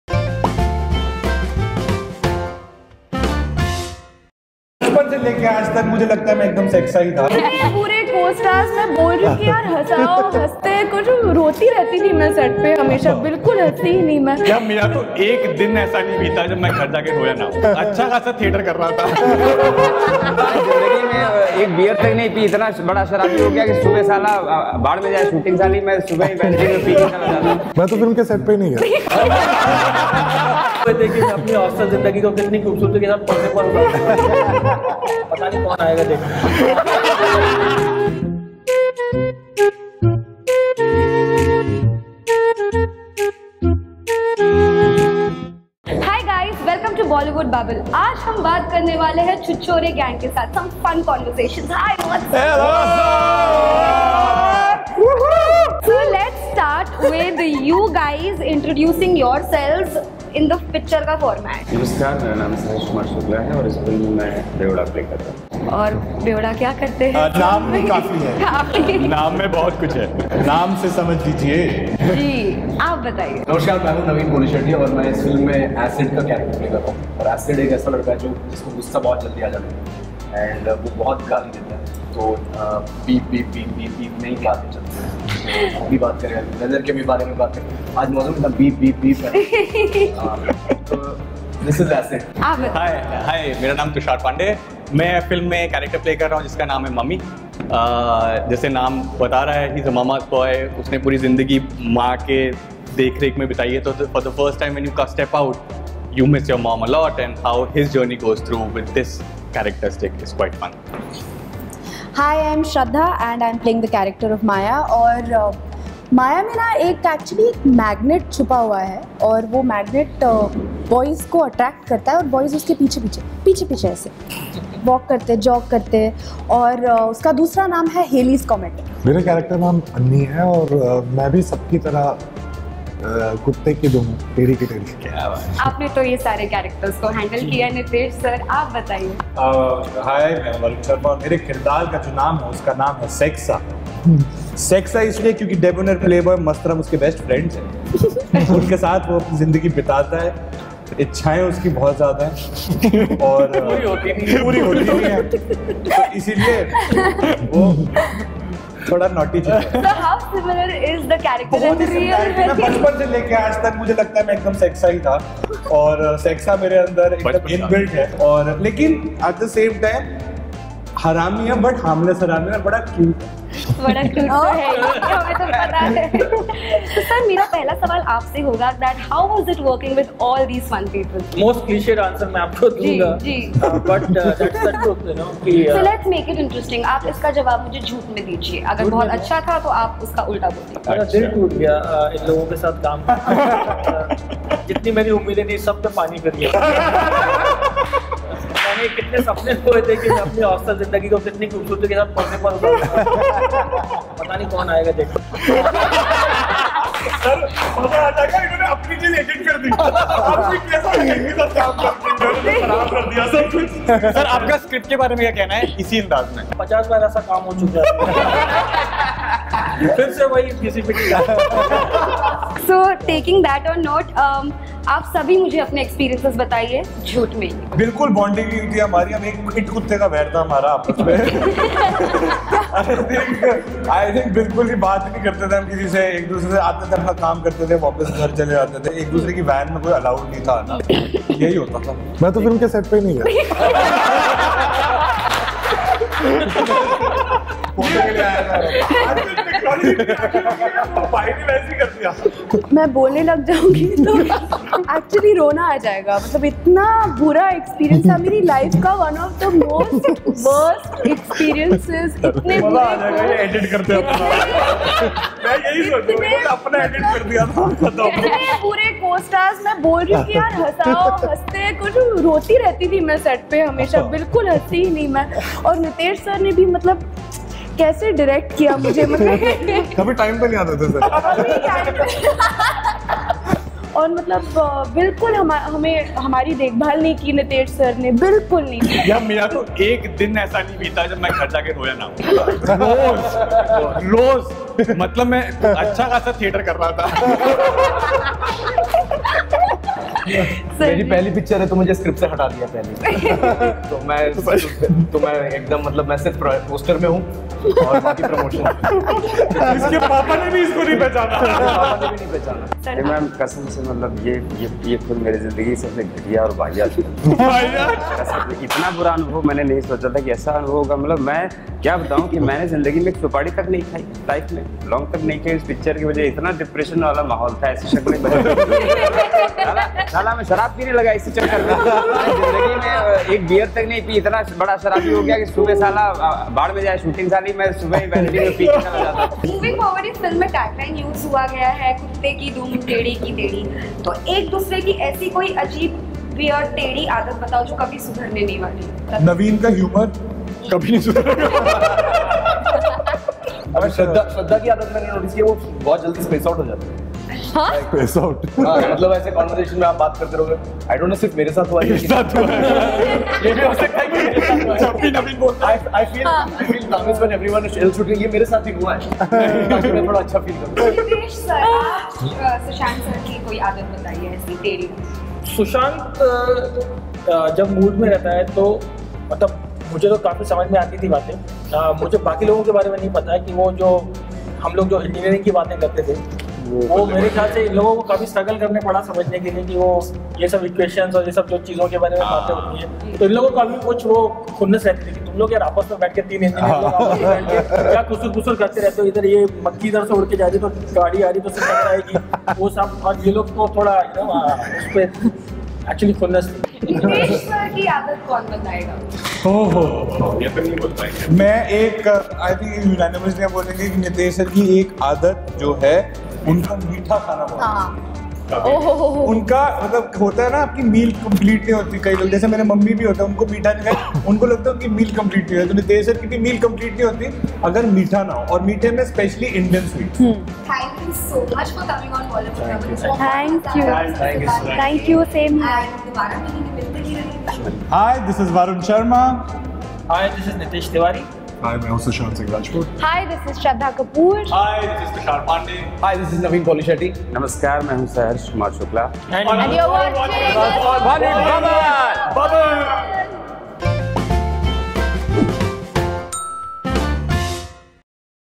CRAZY Conversation With Chhichhore Gang | Sushant Singh Rajput | Shraddha Kapoor | Varun Sharma
Sushant Singh Rajput, Shraddha Kapoor, Varun Sharma and the team 'Chhichhore' have a fun banter in an exclusive interview with Bollywood Bubble, watch it out!